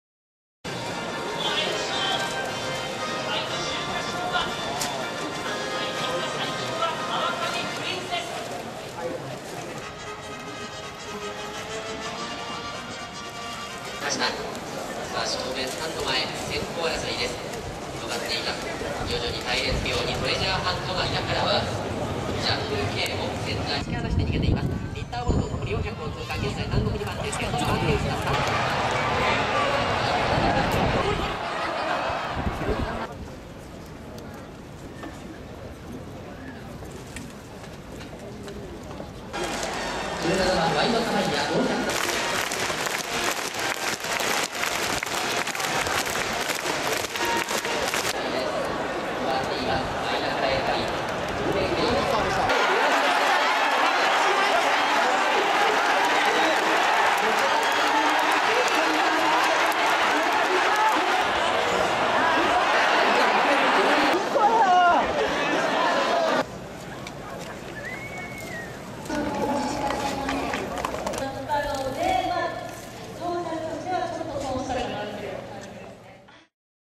馬肥ゆ
日一日と深まりゆく秋。五感を研ぎ澄まし、季節を彩る音を探した。